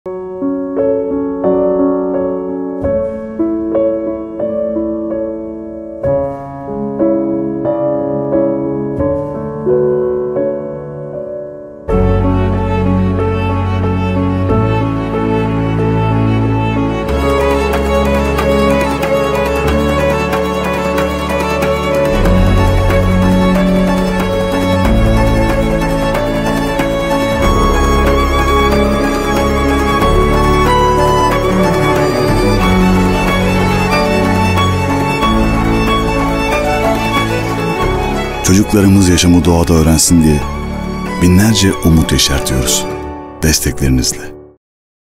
TEMA/Kamu Spotu-3
Vakfın yürüttüğü doğa eğitim programlarına destek çağrısı yapmak amaçlı kamu spotu.